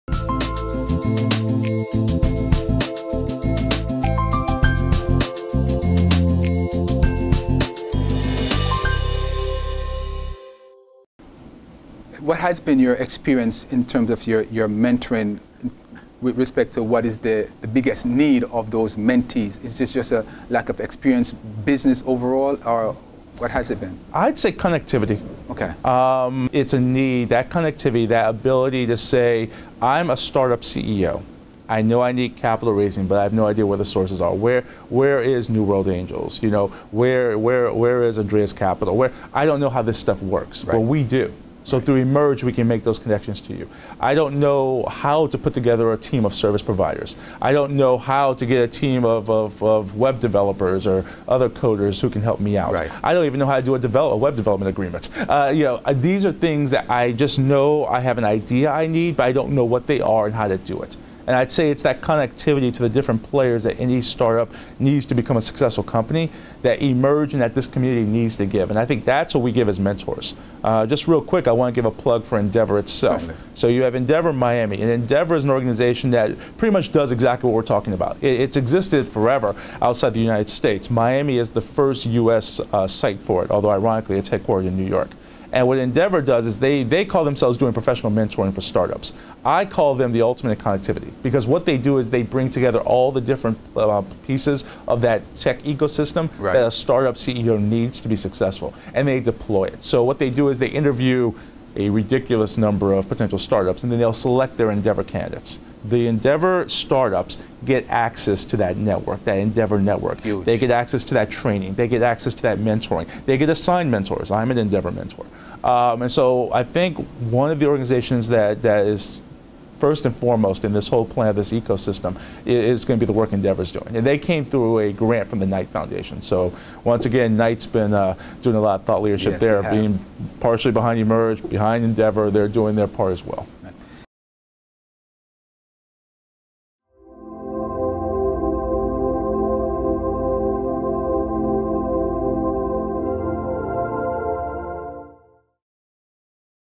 Connectivity: The biggest need of mentees
The value of a mentor/mentee relationship is connectivity says Jaret Davis, Managing Shareholder at Greenberg Traurig (Miami).